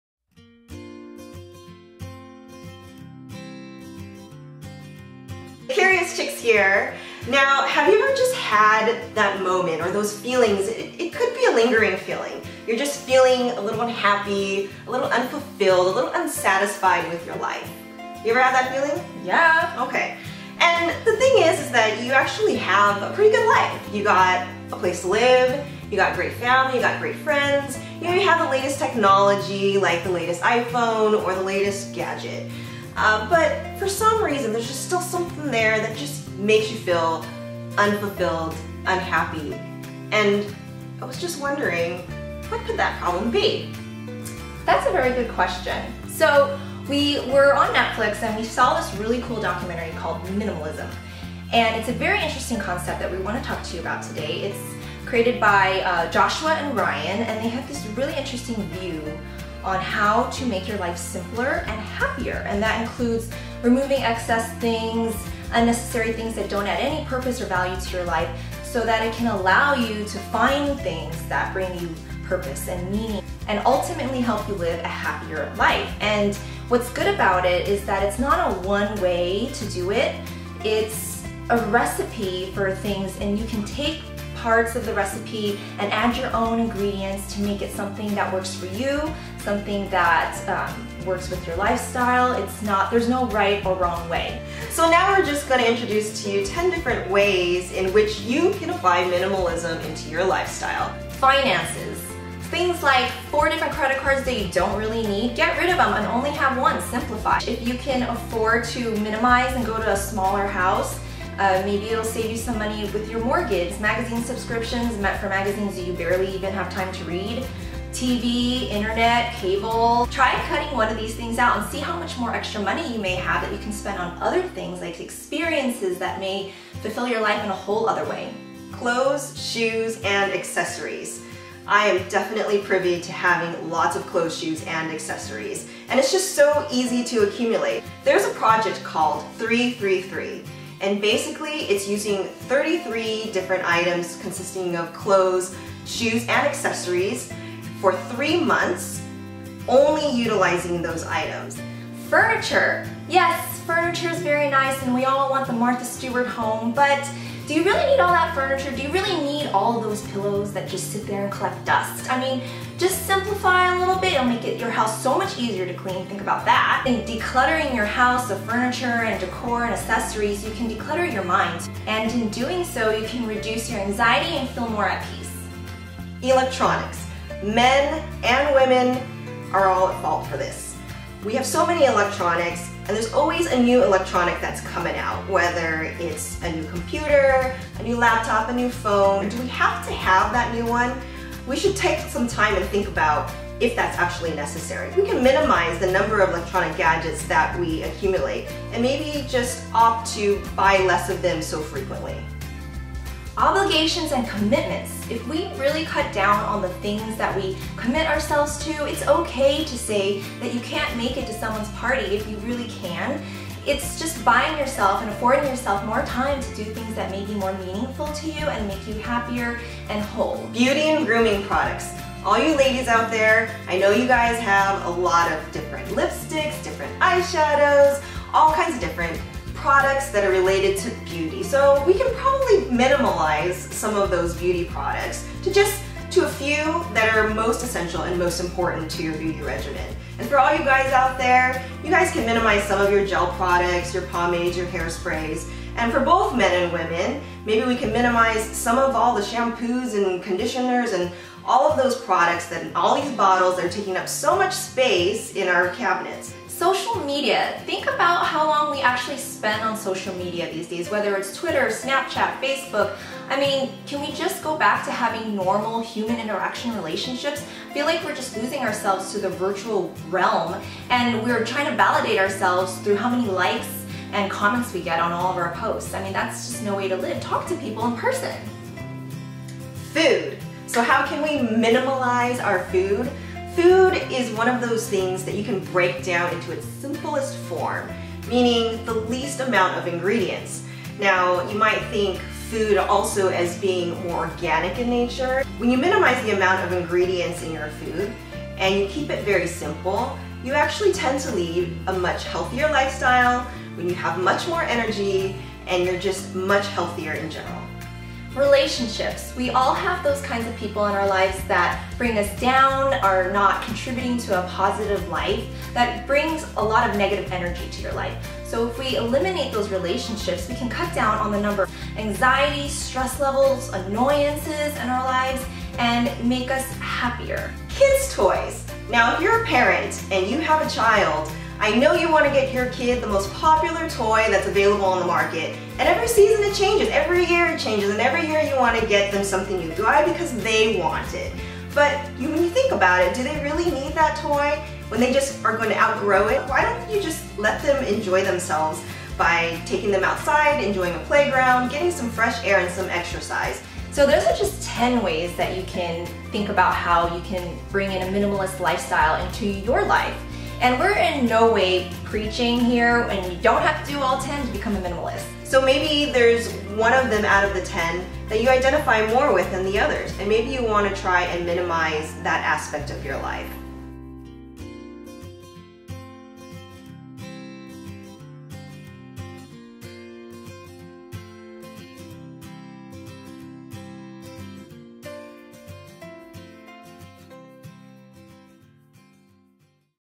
10 Minimalist Living Tips - Minimalism Beginners
Minimalism is a lifestyle in which one minimizes the ‘things’ in our lives that don’t add value or meaning to our lives. But how do we start to live a minimalist lifestyle? The minimalist life doesn’t have to be living with only a mattress and 5 pieces of clothing. In fact you can still have ‘stuff’…but just less of it. Here are 10 minimalist living tips that will help you to start thinking about what areas of your life you’d like to begin.

Many thanks to Joshua Fields Millburn & Ryan Nicodemus, aka The Minimalists for inspiring us to make this video! Here are links to learn more about them and a minimalist lifestyle:

~ Curious Chics